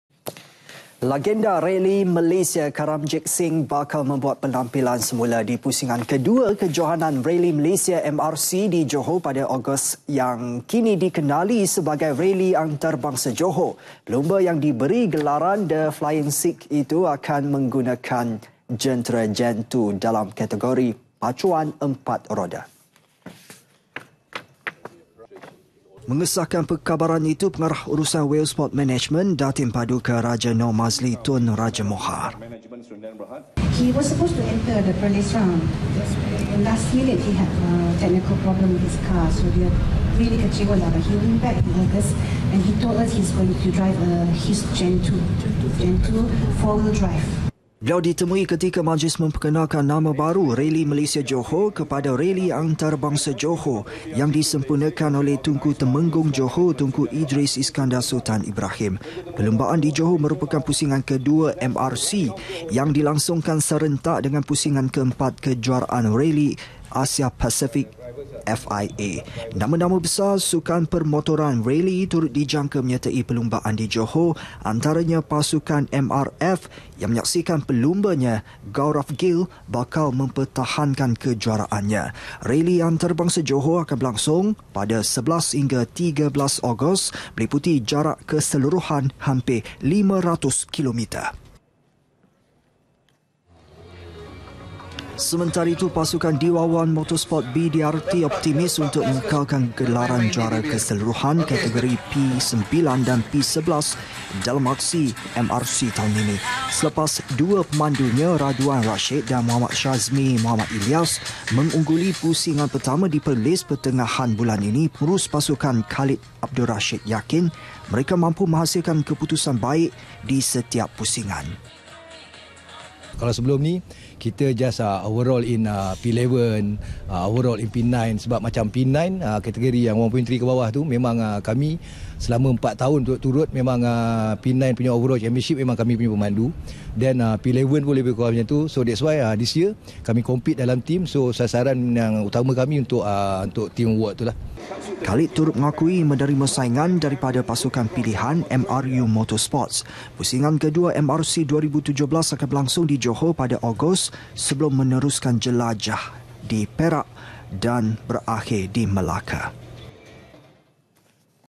RALI ANTARABANGSA JOHOR - KARAMJIT BUAT PENAMPILAN SEMULA [25 MEI 2017]
I. Lagenda rali Malaysia, Karamjit Singh bakal membuat penampilan semula di pusingan kedua, Kejohanan Rali Malaysia (MRC) di Johor, pada Ogos yang kini dikenali sebagai Rali Antarabangsa Johor.

Pelumba yang diberi gelaran The Flying Sikh itu akan menggunakan jentera Gen2 dalam kategori pacuan empat roda.

II. Sementara itu, pasukan Dwau One Motorsport BDRT optimis untuk mengekalkan gelaran juara keseluruhan kategori P9 dan P11 dalam aksi MRC tahun ini.

Selepas dua pemandunya, Roduwan Rashid dan Mohd Shazmie Mohd Ilias mengungguli pusingan pertama di Perlis pertengahan bulan ini , Pengurus pasukan Khalid A.Rashid yakin, mereka mampu menghasilkan keputusan baik di setiap pusingan.